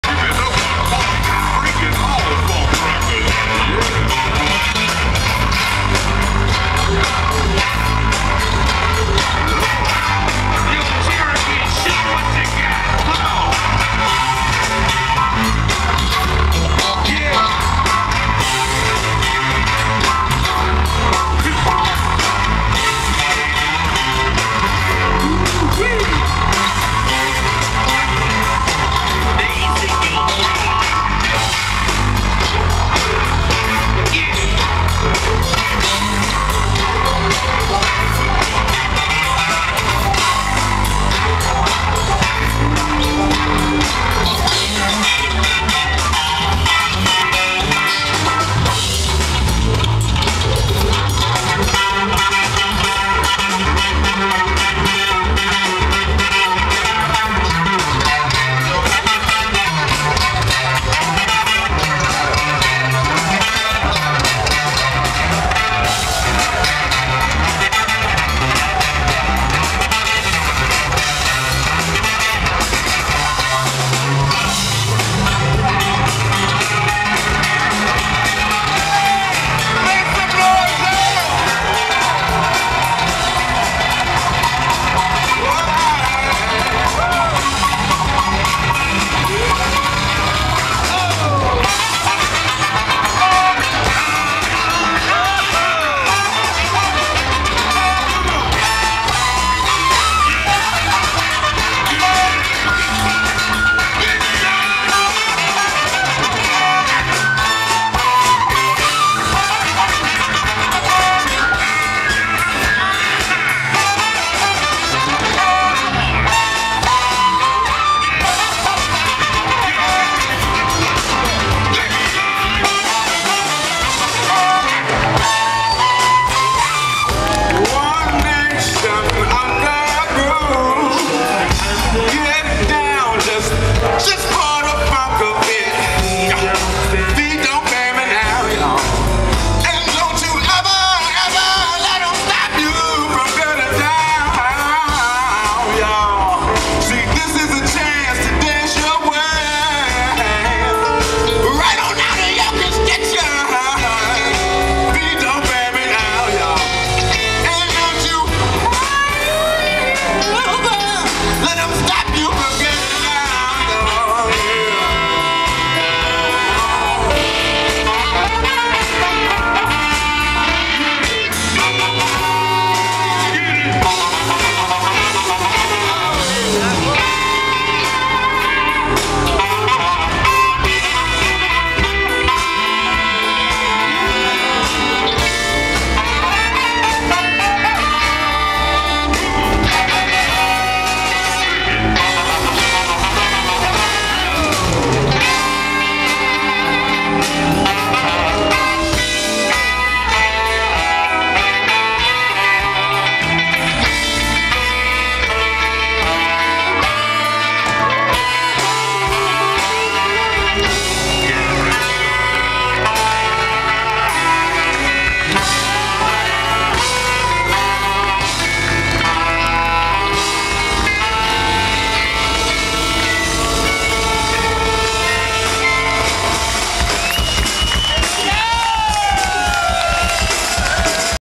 George Clinton, Funkadelic & Parliament Festival Cultura Inquieta Getafe 17/07/2014
3 horas de concierto 3 en la plaza de toros de Getafe en un Festival muy bien armado y acogedor (Cultura Inquieta). Fiesta P-Funk asegurada cuando toca el creador de este estilo. Con "solo" 17 músicos sobre el escenario. A los que hemos tenido oportunidad de verle en otras ocasiones con aún más músicos  y en otrs épocs sigue fiel a su espíritu libre a pesar de que haya cambidado su imagen y extravagancia un poco. ¡MEMORABLE!